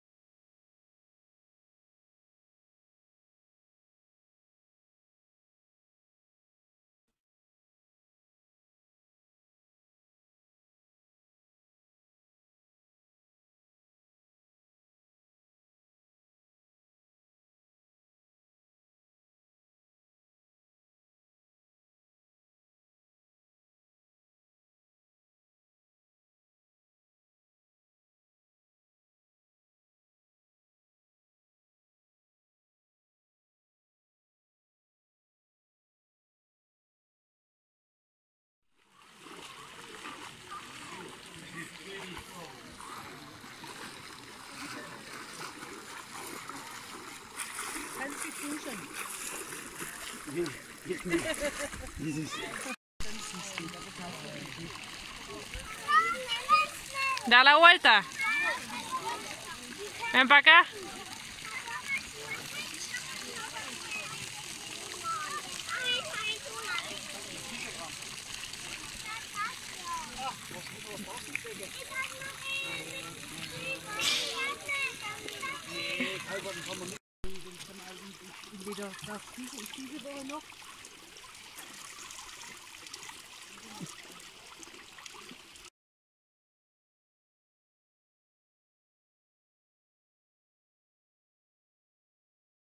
TOMAS FALSAS...LO QUE NO SALE EN LOS VÍDEOS
A veces cuando grabamos no todo sale a pedir de boca, no todo sale bien y no aparece todo lo que ocurre. Se trata de concentrar lo mejor en sólo un vídeo. Hoy traigo un vídeo demostración en una de las subidas más duras que he echo nunca...el Ahornspitze. Los Alpes autríacos son una maravilla y cada vez que vengo por aquí quedo maravillado. Con un poquito de suerte volveremos a rodar por aquí.